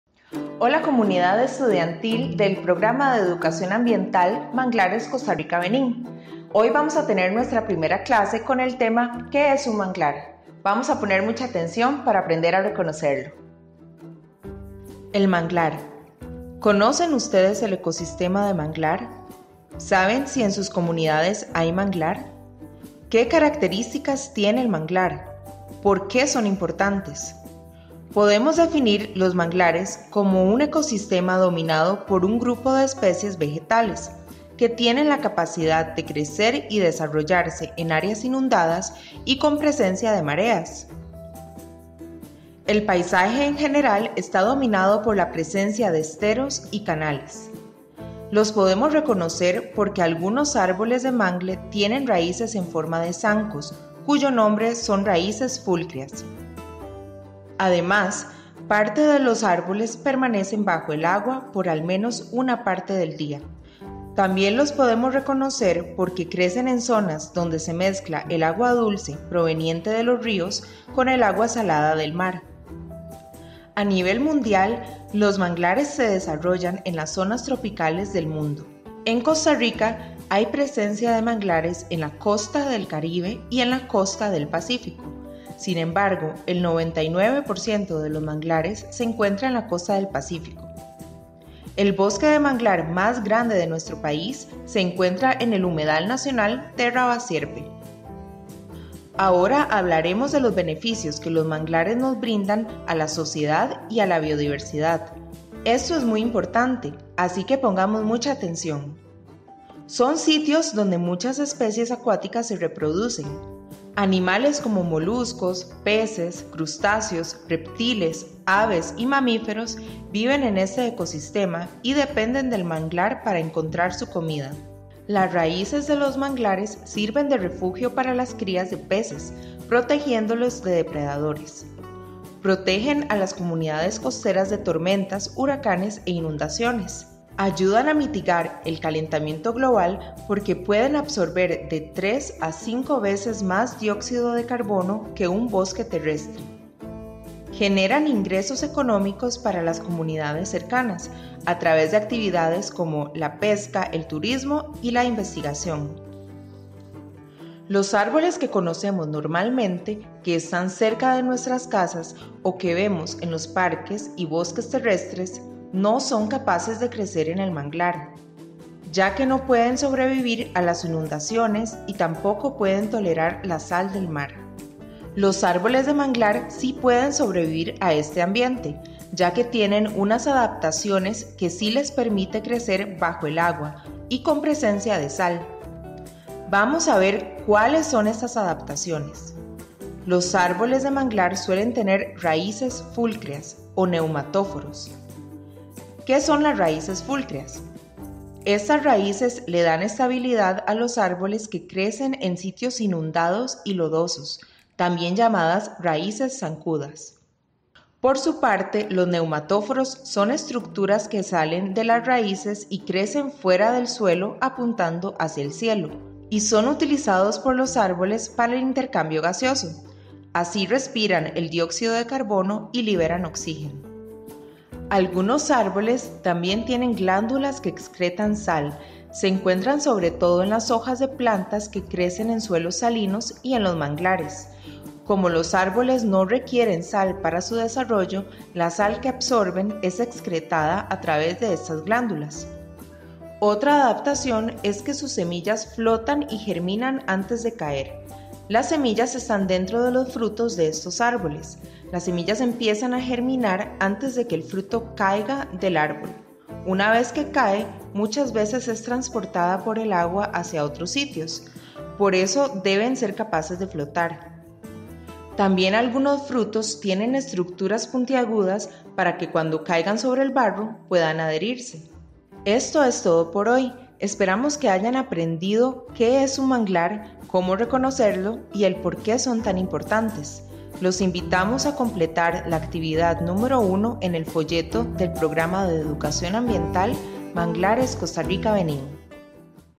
Clase 1: ¿Qué es el Manglar?
Esta clase nos explica Qué es un Manglar, su importancia y sus características. Esperamos que lo disfruten.